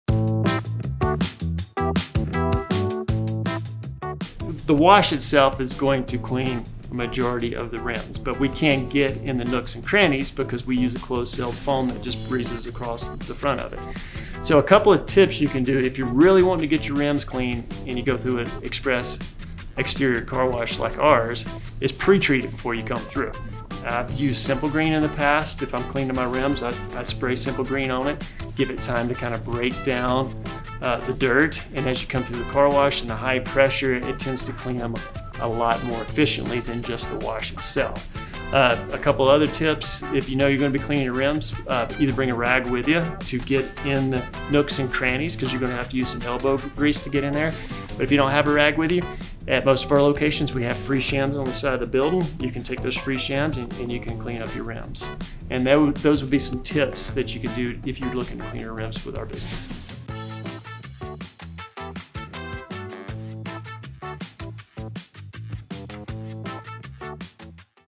How Do I Clean My Rims at The Car Wash?
Struggling to get those rims spotless? Discover insider tips to achieve perfectly clean rims, even in those hard-to-reach nooks and crannies! Dive into the magic of pre-treating, learn about the effectiveness of Simple Green, and grab a bonus tip on using our free shams at Breeze Thru Car Wash locations. Clean rims have never been this easy! 🚗✨